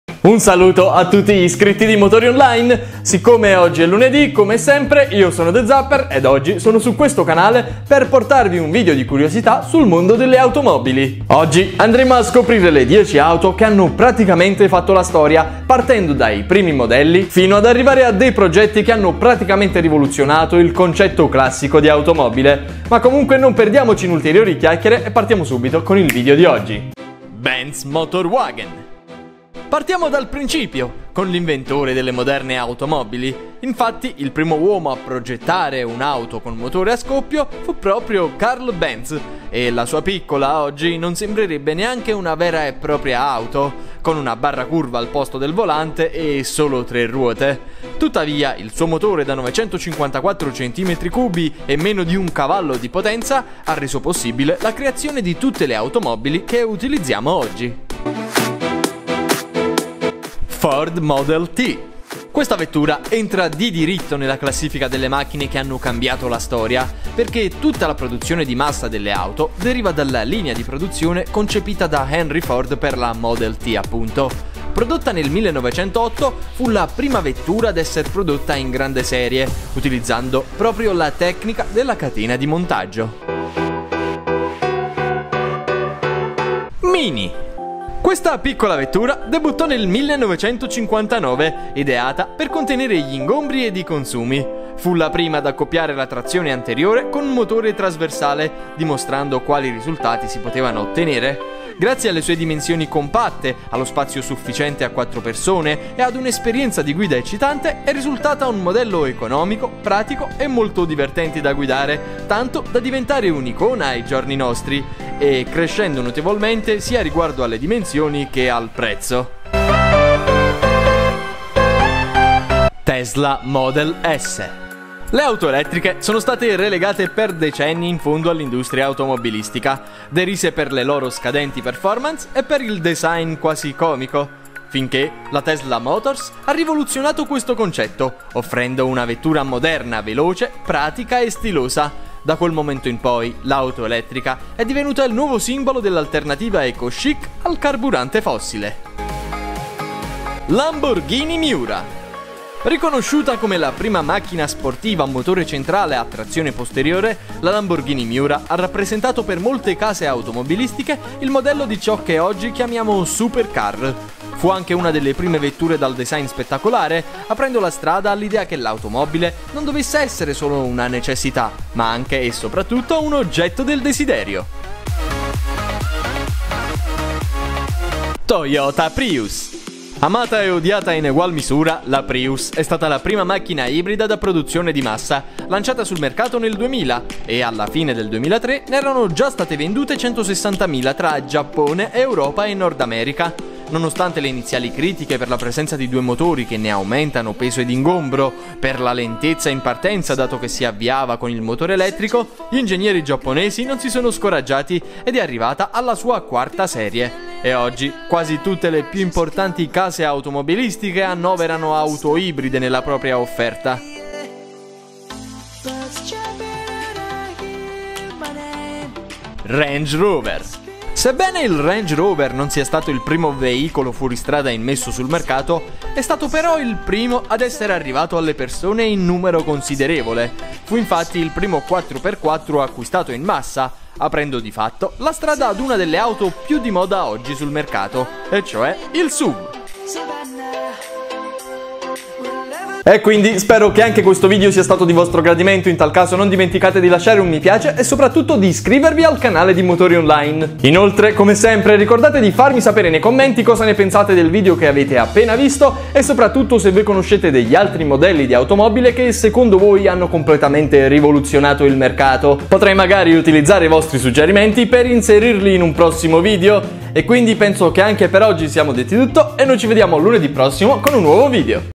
Le 10 AUTO che hanno cambiato la STORIA | The Zapper
Tra le 10 auto che hanno cambiato la storia troviamo la Benz Motorwagen la prima auto costruita al mondo, la Ford Model T con la quale venne inventato il sistema della catena di montaggio, La mini minor piccola ed economica, La Tesla model T prima auto elettrica ad altre prestazioni, la Lamborghini Miura prima supercar al mondo, la Toyota prius, prima auto ibrida immessa sul mercato e la Rage Rover che ha aperto il mercato per i SUV